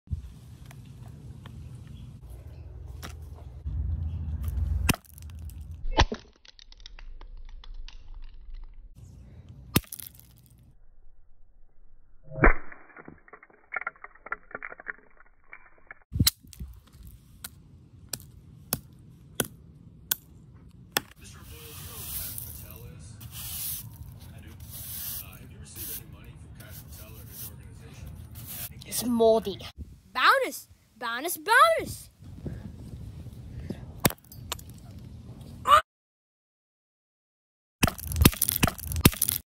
Smashing a nut!
I found a squirrel nut in plain sight on a bench, and I took it. Later my mom found another squirrel nut but it was cut in half! I thought to break it because... well I felt like it.